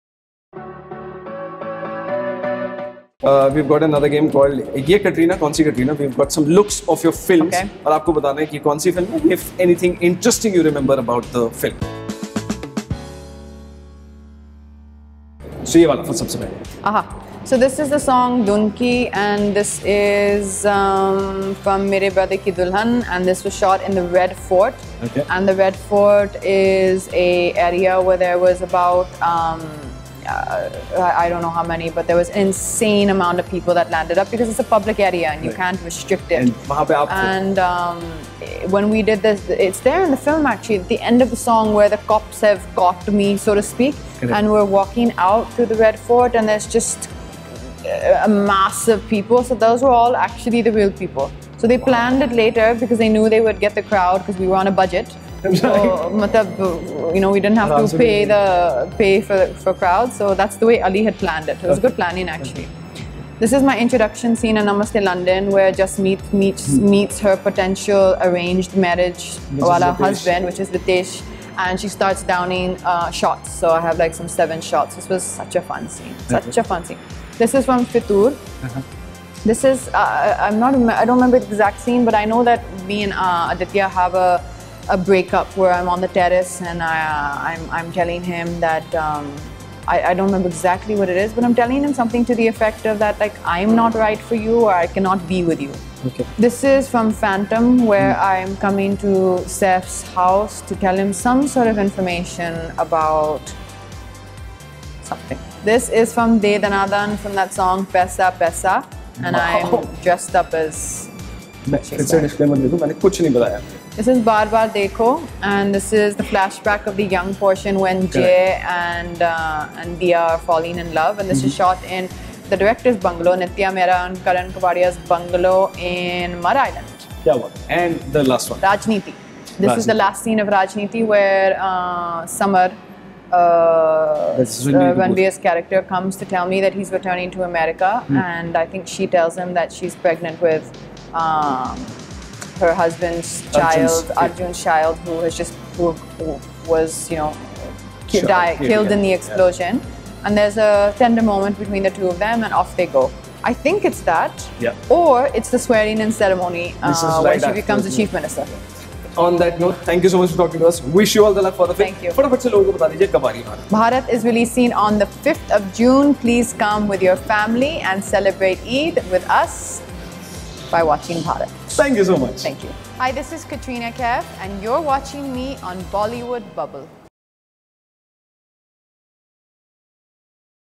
'Bharat' star Katrina Kaif Plays The HILARIOUS Game Of 'Yeh Katrina Kaun Si Katrina'
Ahead of 'Bharat' release Katrina Kaif plays the fun game of 'Yeh Katrina Kaun Si Katrina', where she is shown old pictures of her films and let's see if she is able to guess them all.